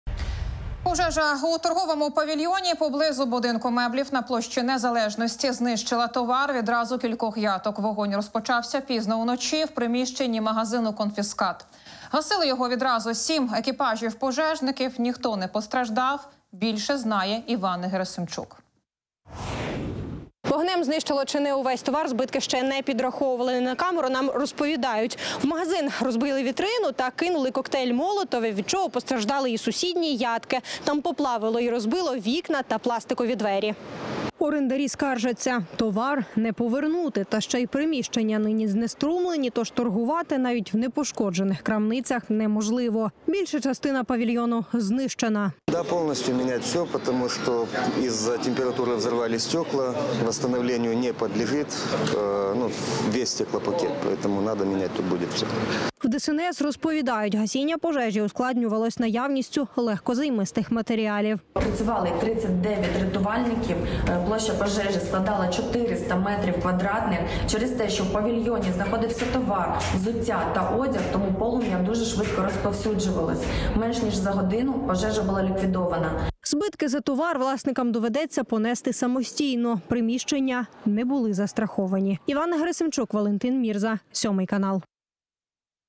Пожежа у торговому павільйоні на площі Незалежності
Пожежа у торговому павільйоні поблизу Будинку меблів на площі Незалежності знищила товар відразу кількох яток. Вогонь розпочався пізно уночі в приміщенні магазину «Конфіскат». Гасили його відразу сім екіпажів пожежників. Ніхто не постраждав.

«Вогнем знищило чи не увесь товар. Збитки ще підраховують. Не на камеру нам розповіли: в магазині розбили вітрину та кинули “коктейль Молотова”, від чого постраждали і сусідні ятки. Там поплавило й розбило вікна та пошкодило пластикові двері», — розповіла кореспондент 7 каналу Іванна Герасимчук.

Орендарі скаржаться: товар не повернути. Та ще й приміщення нині знеструмлені, тож торгувати навіть в непошкоджених крамницях неможливо. Більша частина павільйону знищена.

У ДСНС розповідають: гасіння пожежі ускладнювали легкозаймисті матеріали.

Збитки за товар власникам доведеться понести самостійно. Приміщення не були застраховані.